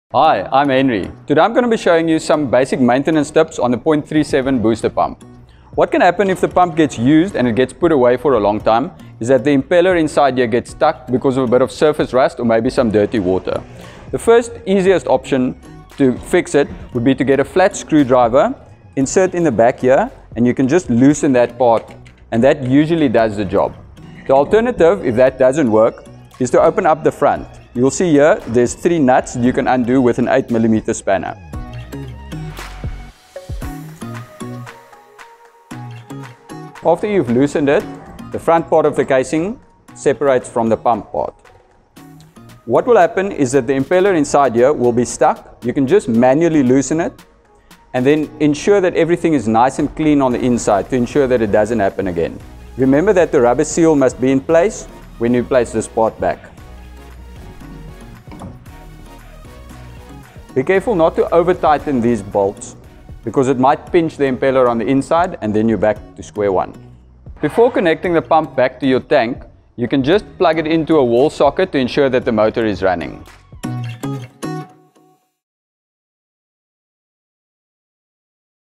DIY with JoJo_Maintaining your 0.37kW Booster Pump
Avoid unnecessary breakdowns and keep your 0.37kW JoJo Booster pump running smoothly with these easy maintenance steps.

For our detailed user manual and usage guidelines, visit jojo.co.za.